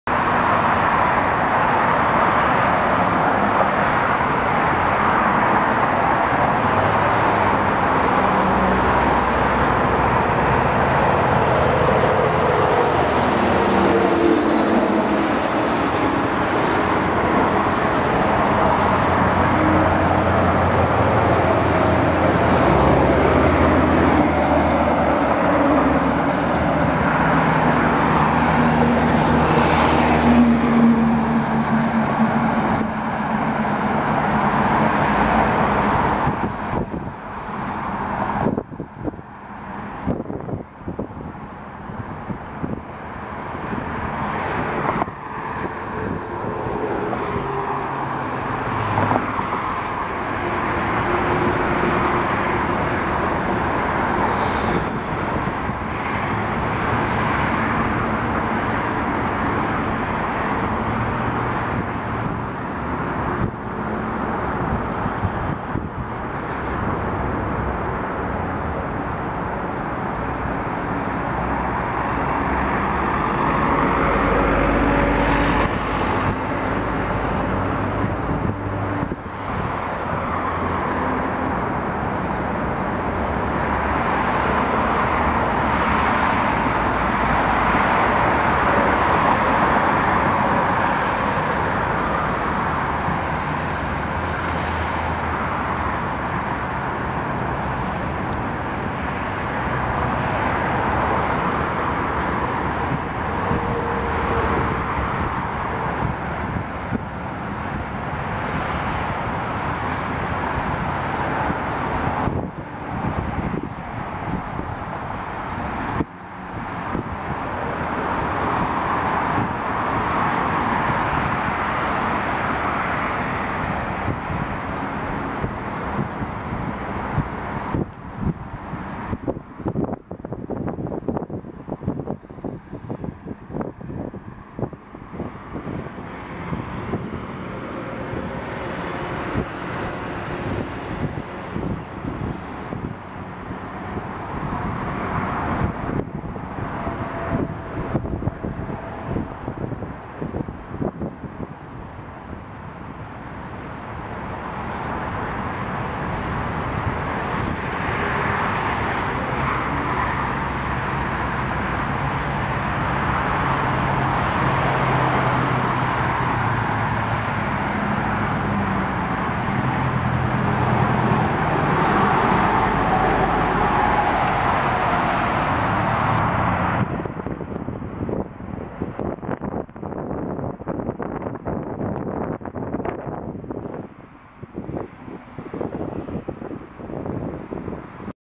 617 Martin Goodman Trail Budapest Park to Marilyn Bell Park 5
Continuing our walk on the Martin Goodman Trail at Budapest Park and ending at Marilyn Bell Park. The trail is part of the Waterfront Trail with Lake Ontario to the south of the trail.